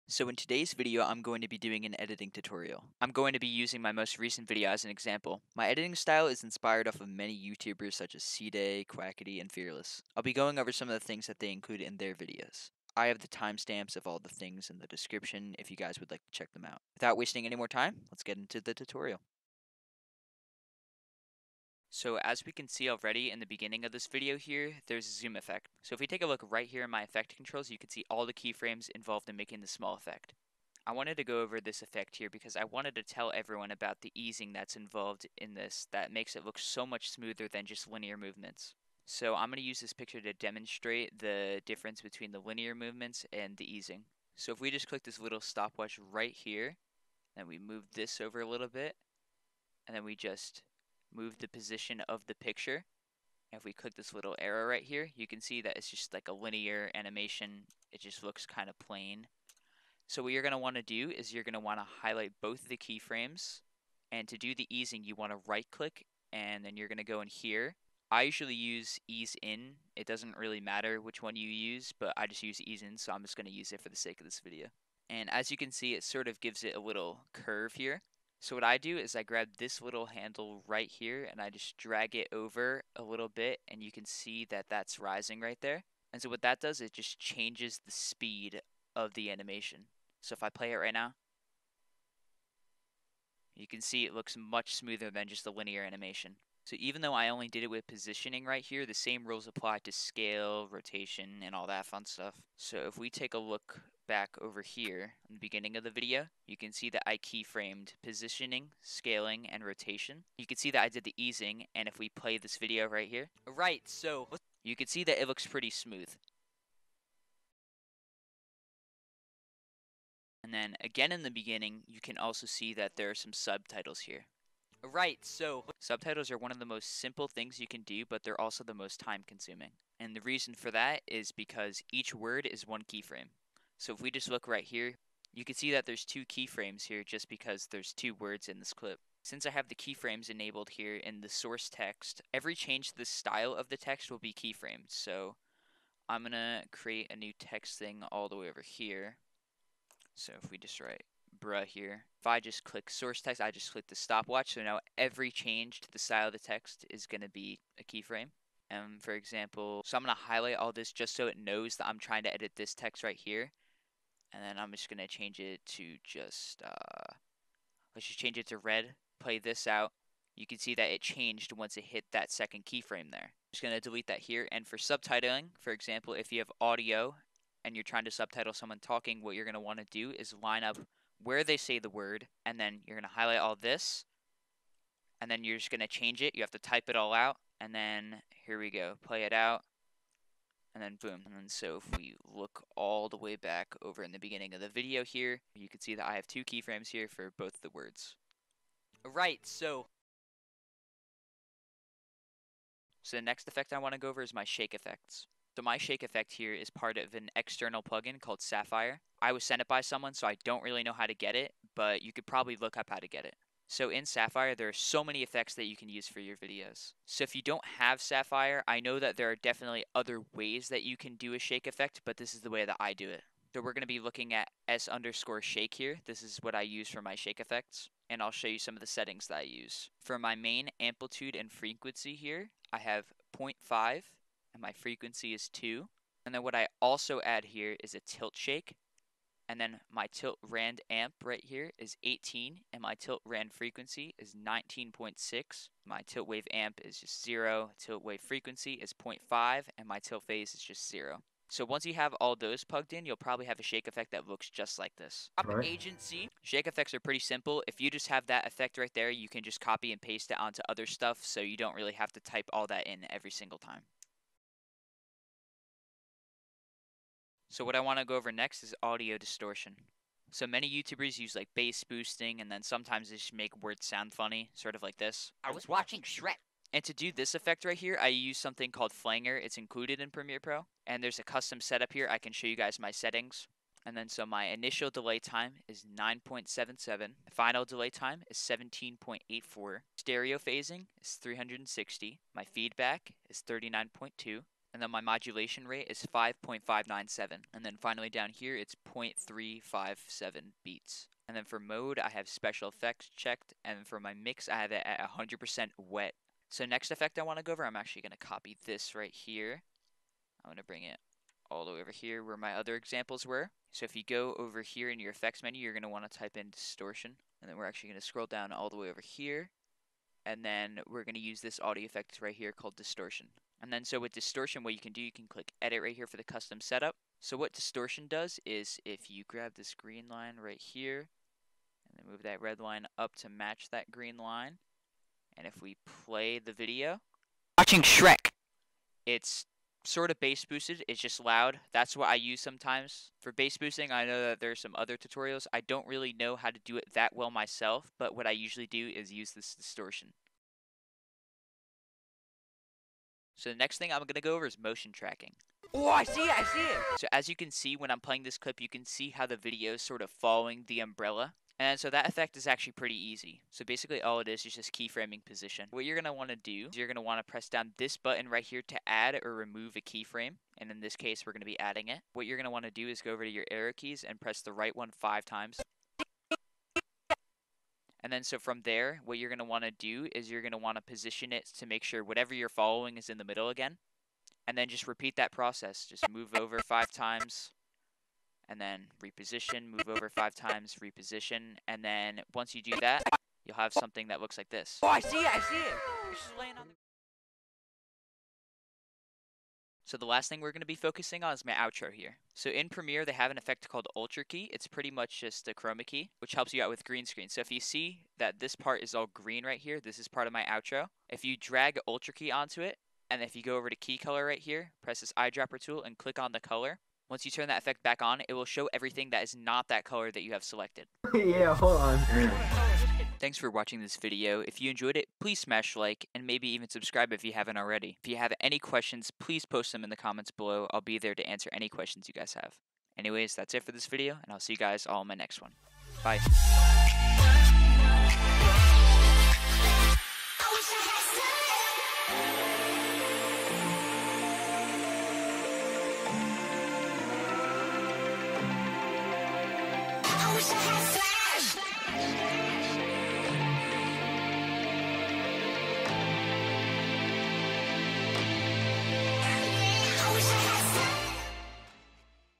How to Edit Like Ceeday, Quackity, Fearless in Premiere Pro! (Editing Tutorial)
Wow, double upload.

TIMESTAMPS
Zoom Effects - 0:20
Subtitles - 1:58
Shake Effects - 3:20
Audio Distortion - 4:25
Motion Tracking - 5:51
Greenscreening - 6:37

Use Code: FuzzySucks in the Item Shop and In The Epic Games Store!

I hope you guys enjoy the video! :D